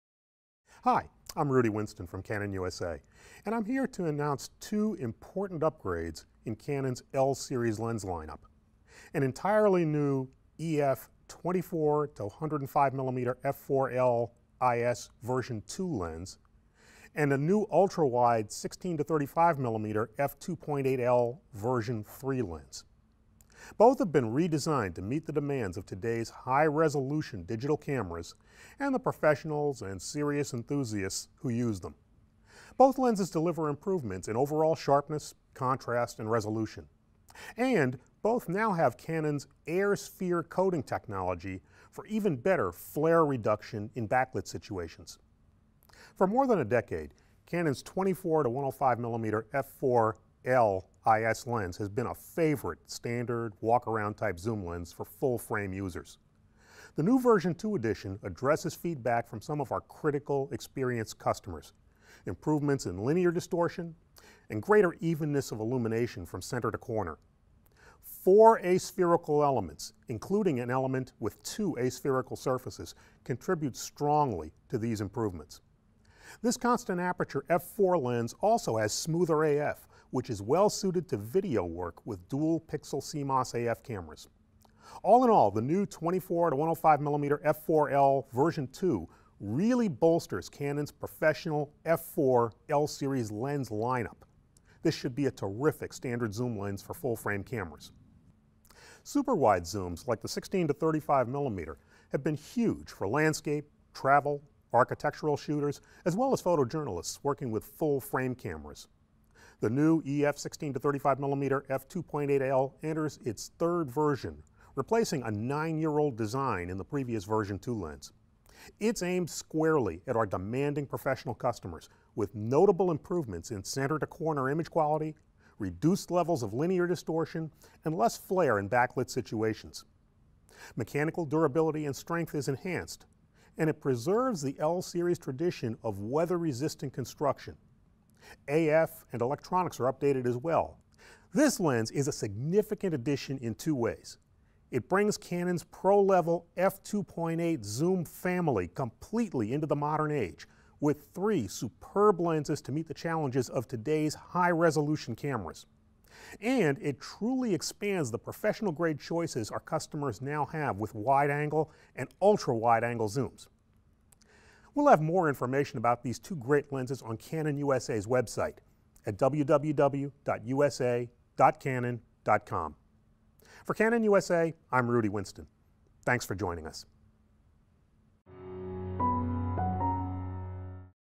Canon EOS - Introducing the new Canon EF 24 105mm f4L IS II USM and EF 16 35mm f2 8L III USM Lenses
The EF 16-35mm f/2.8L III USM is an indispensable ultra wide-angle zoom lens for professional and enthusiast photographers who demand superlative operation and performance, in virtually any condition. Its 16-35mm focal range provides superb control of perspective, helping you capture pictures and movies with amazing, dramatic compositions limited only by your imagination. Combining outstanding image quality, a fast, constant aperture and a rugged design, the EF 16-35mm f/2.8L III USM delivers eye-catching results whether capturing detailed, nuanced landscapes, dramatic interiors and much more. Both still and moving images can be captured with consistent edge-to-edge sharpness, improved peripheral illumination and reduced distortion at extreme wide angles - especially important when using SLR cameras with full-frame sensors. Engineered with high-resolution photography and videography in mind, the EF 16-35mm f/2.8L III USM provides the optical performance necessary for critical applications, while delivering the renowned function and feel you've come to expect from Canon's celebrated L-series lenses.

EF 24-105mm f/4L IS USM

This easy-to-use standard zoom lens can cover a large zoom area ranging from 24mm wide-angle to 105mm portrait-length telephoto, and its Image Stabilizer Technology steadies camera shake up to three stops. Constructed with one Super-UD glass element and three aspherical lenses, this lens minimizes chromatic aberration and distortion. The result is excellent picture quality, even at wide apertures. Canon's ring-type USM gives silent but quick AF, along with full-time manual focus. Moreover, with dust- and moisture-resistant construction, this is a durable yet sophisticated lens that meets the demands of advanced amateur photographers and professional photographers alike.